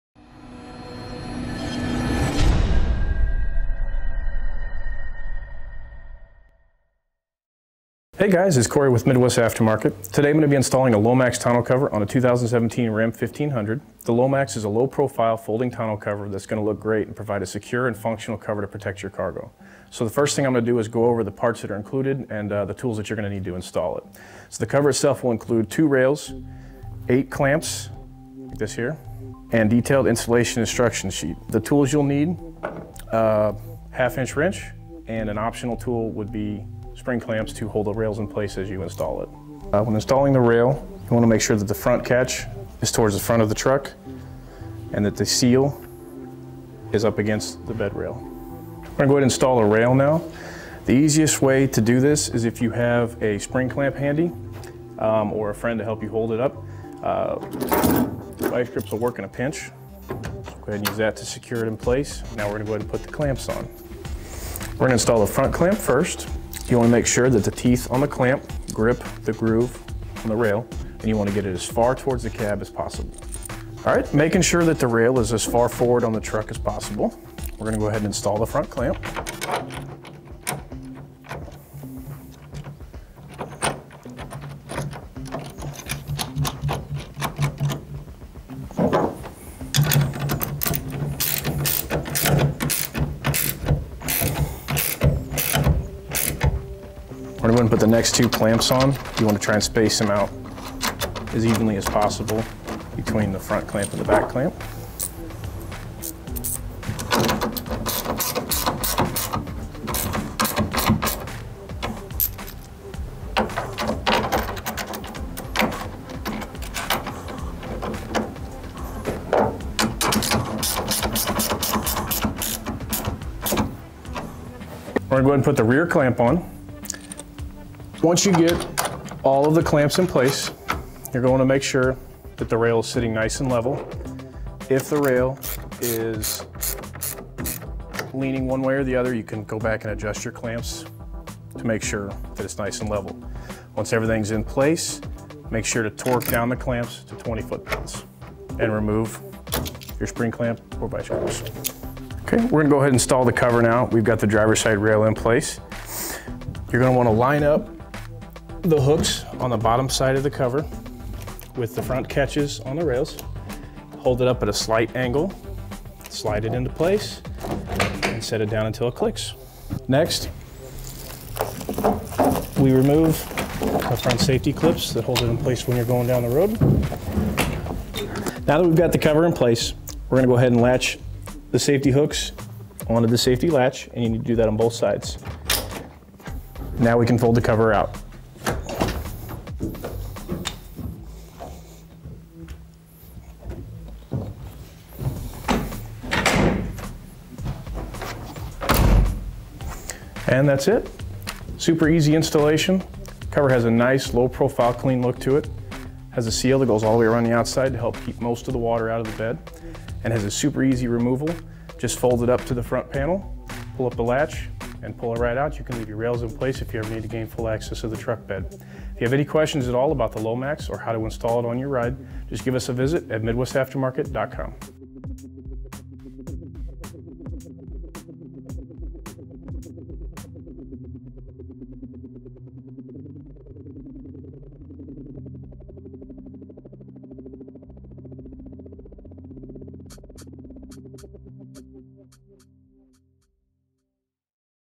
Lomax Tri Fold Tonneau Cover Install
Learn from the best as Cory shares just how easy the Lomax Tri Fold Tonneau Cover is to install on a 2017 Dodge Ram 1500. The Lomax is a low profile tonneau cover that will not only keep your cargo safe but look good doing it as well.